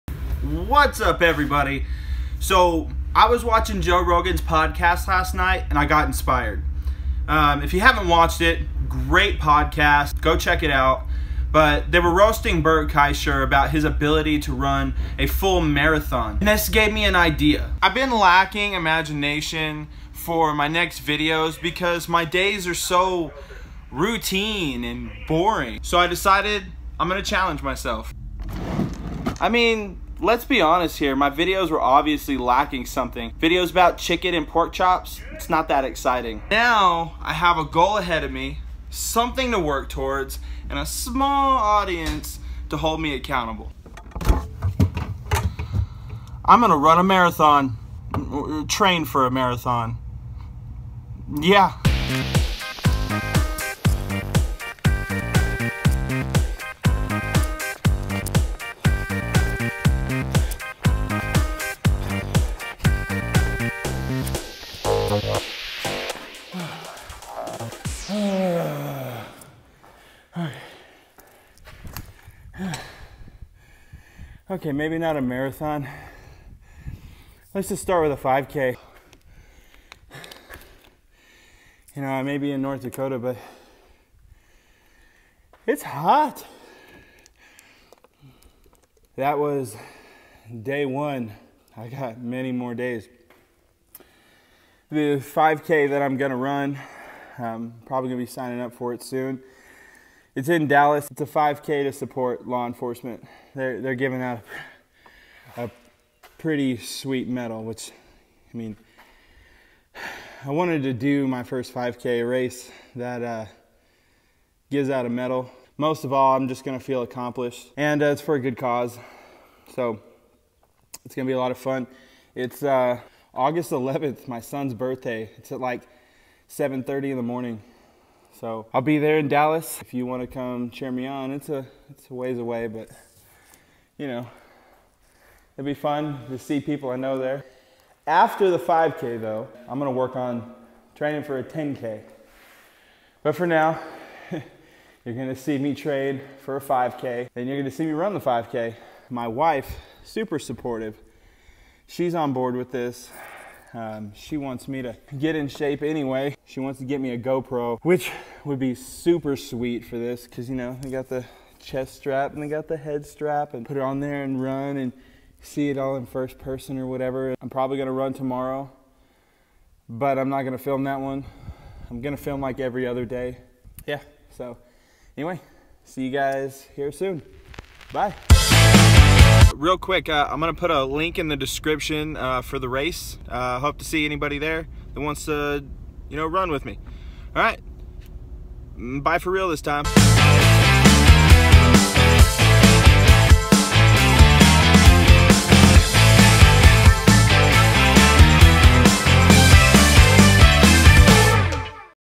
Fat to Fit 5k 1 | Vlog 3
Found some inspiration, grabbed my shoes and my camera! Going to tell you about my new goal I’ve laid out for myself! Going to work hard and do something great!